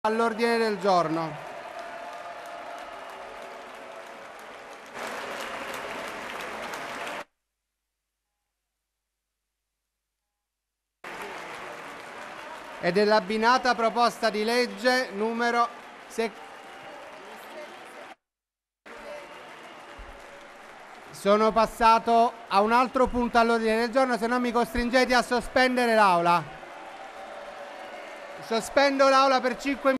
Approvata la Legittima Difesa, applaudono tutti eccetto i deputati M5S
(Agenzia Vista) - Roma, 6 Marzo 2019 - La Camera dei Deputati ha approvato il testo sulla legittima difesa. 373 i voti favorevoli provenienti dal centrodestra e da buona parte del Movimento 5 Stelle.